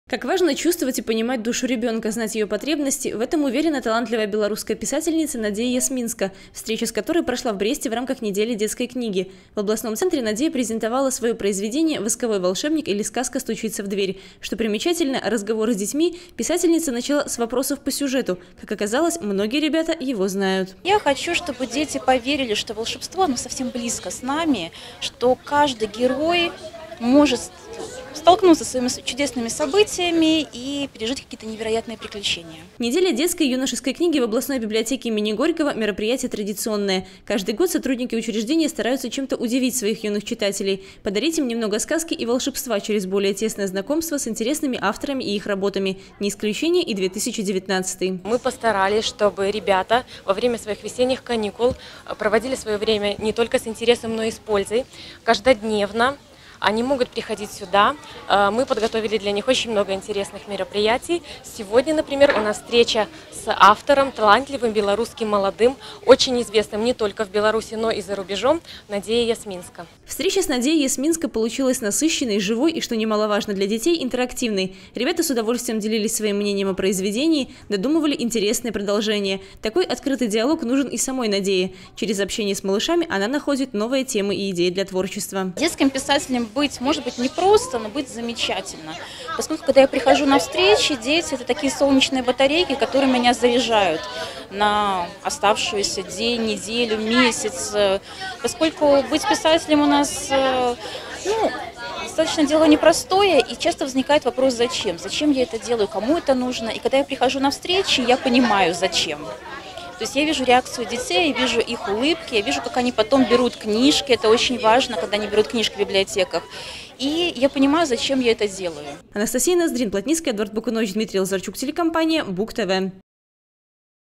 2019-03-29 г. Брест. Творческий вечер Н. Ясмински. Новости на Буг-ТВ.
Сегодня среди взрослых бытует мнение о том, что дети не хотят читать книги, мол, только гаджеты им подавай. Однако это не так, уверена белорусская писательница Надзея Ясминска. Уже не один год произведения на русском и белорусском языках этого талантливого автора довольно быстро разлетаются с прилавков магазинов. А то, что книги девушки читаемы и любимы детьми, подтвердила очередная встреча, которая прошла в Бресте на базе областной библиотеки имени Горького. Здесь писательница презентовала свою новую творческую работу, а также ответила на вопросы юных читателей.